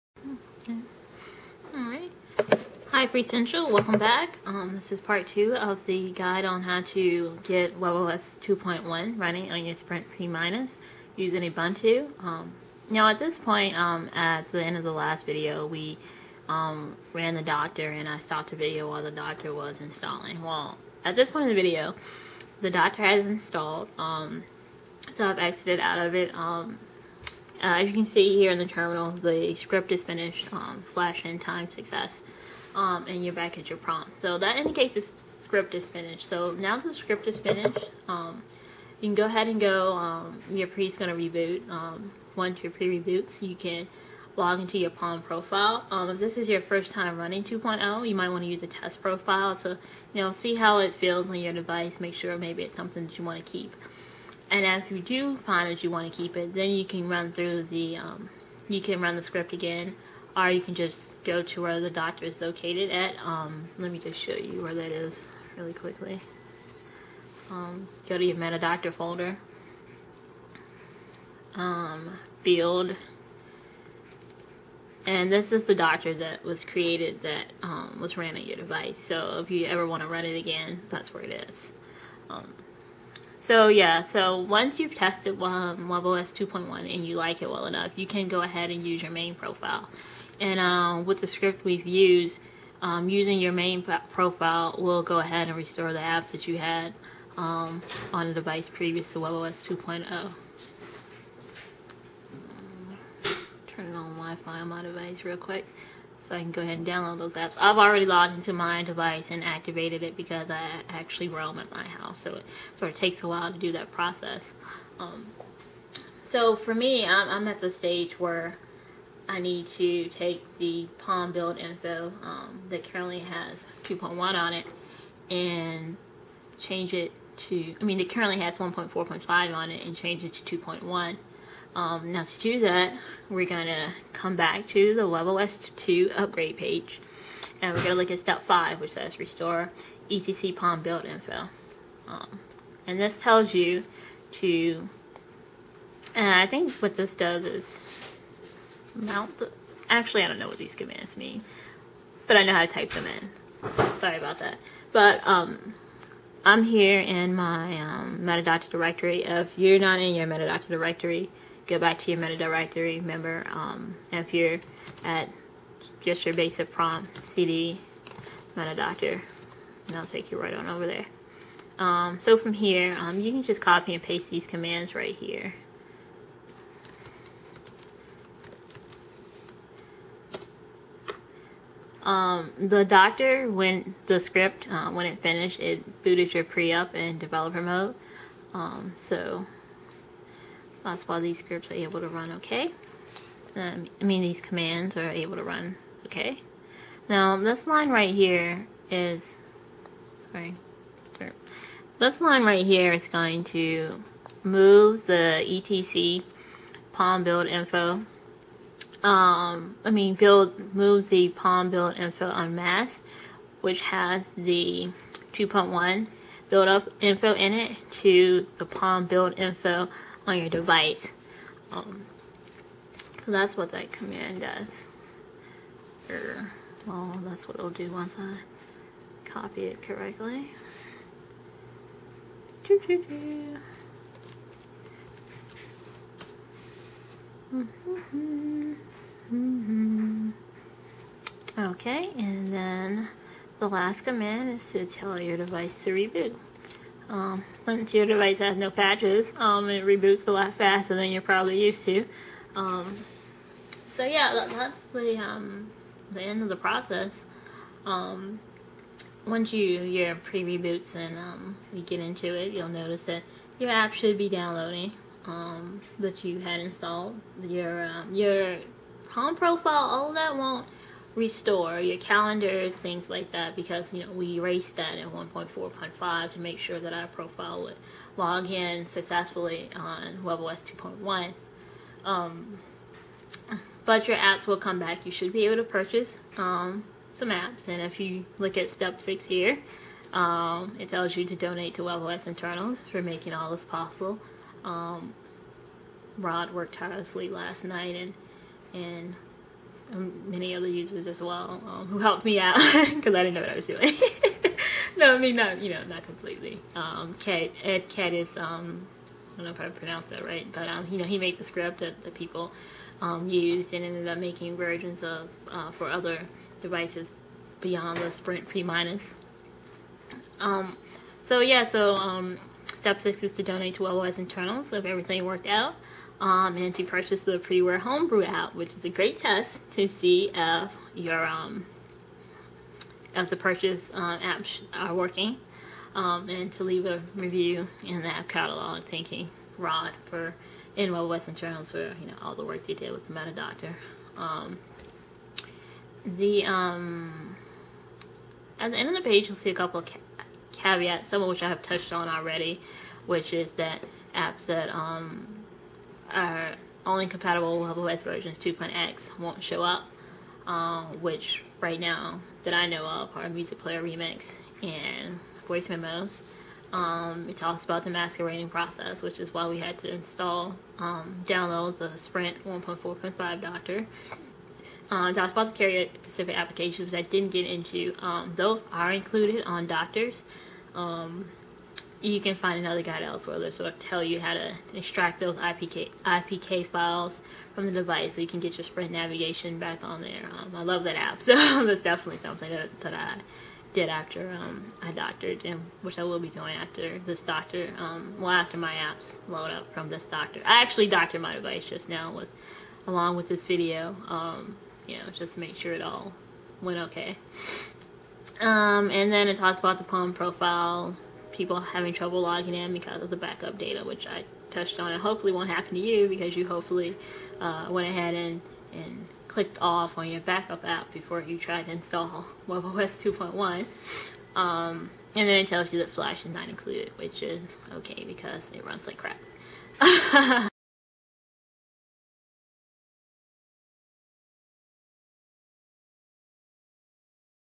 How To Install webOS 2.1 on a Sprint Pre Using Meta-Doctor on Ubuntu (Part 2/2)
To check your palm profile version:

and log in with your palm profile login information. Your profile should say 1.40.50 (for those who had to masquerade) and 2.1.0 for those (Sprint/O2) who did not. If it says 9.9.9, something went wrong and your catalog will not work as expected.

CDMA People: To check firmware, go to Device info, scroll down and click the More Info button. You should see listed beside firmware:

RC1.7(557)